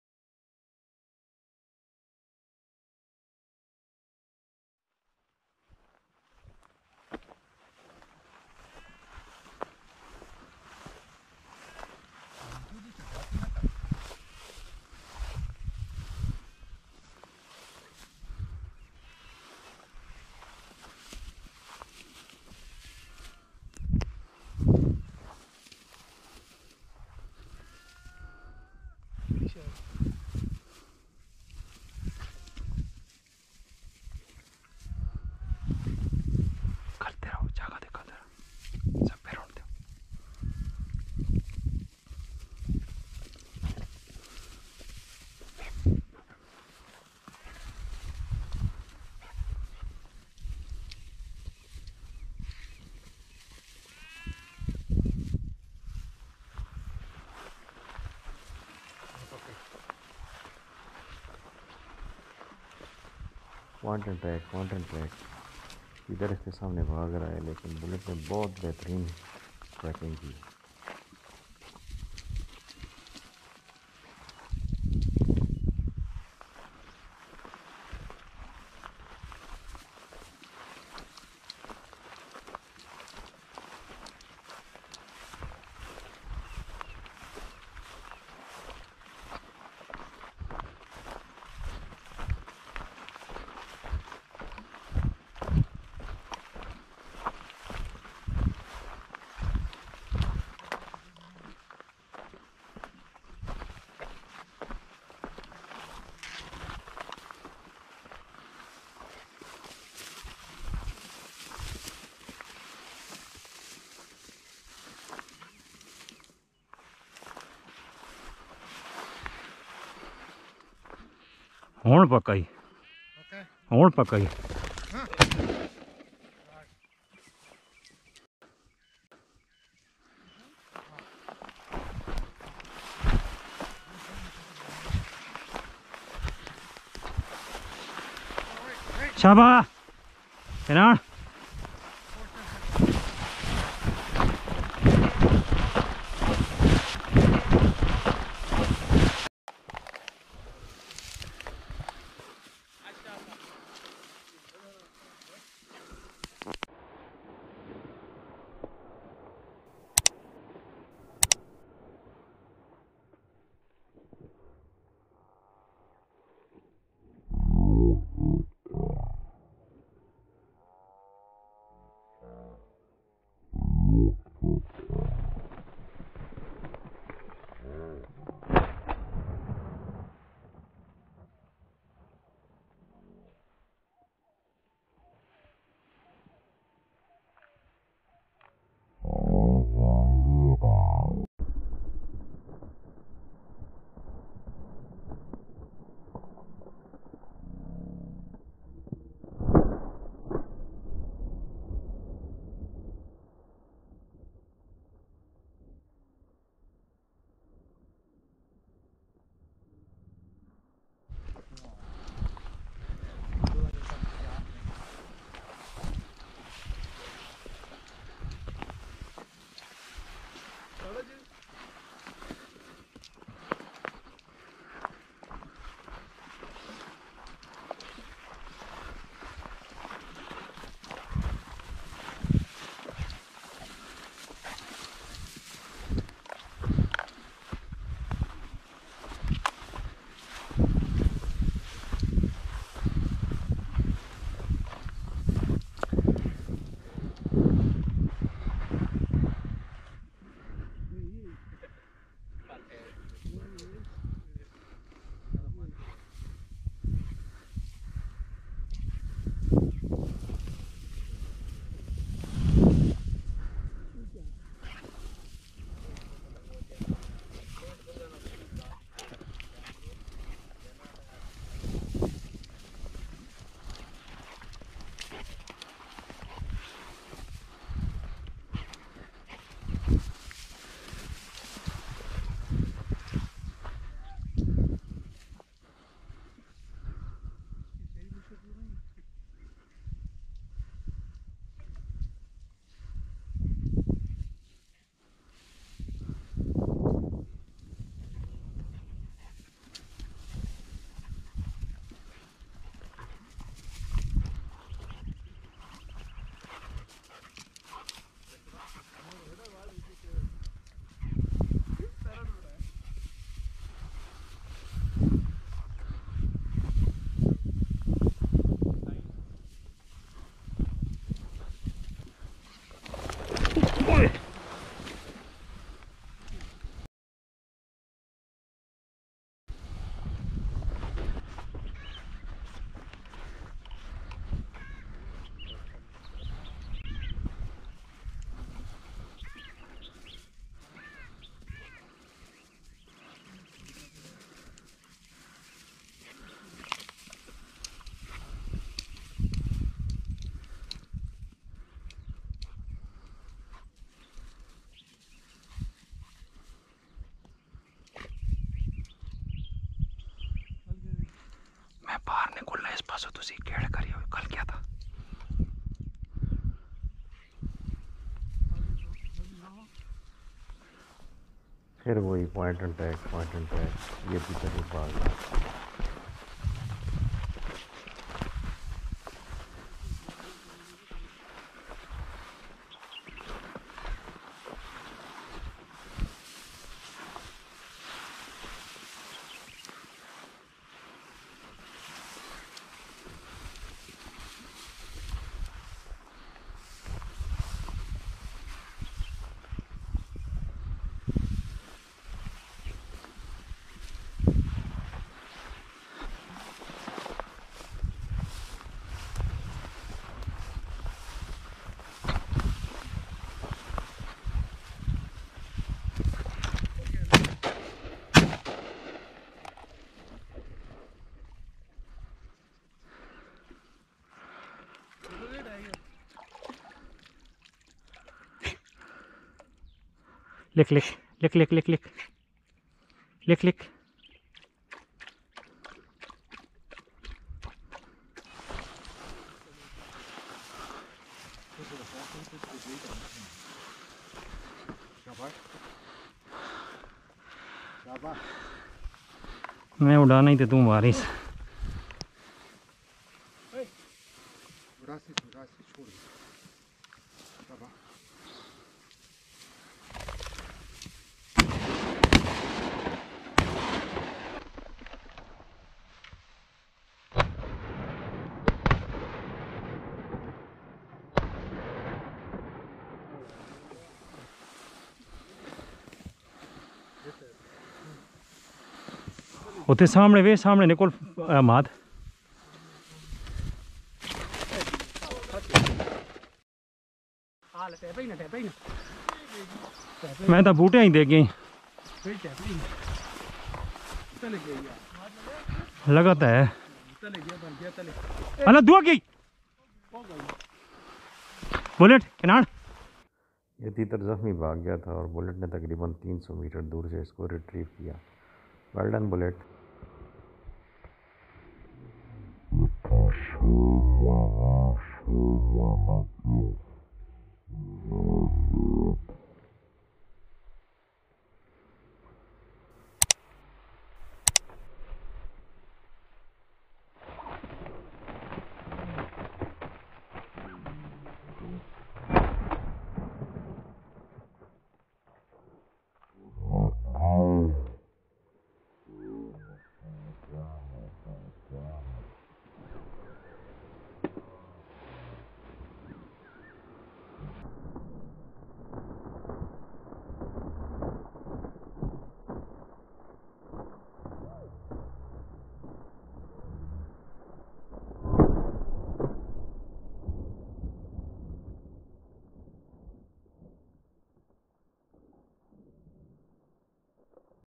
Partridge hunting in Pakistan (the best field work by bullet so far)
Partridge hunting in Pakistan
5 Dec, 2021
gun Baikal mp-155

Cartridge Wah industries bird shot
Cam
Gopro Hero 8 black
my lovely Bullet German shorthaired pointer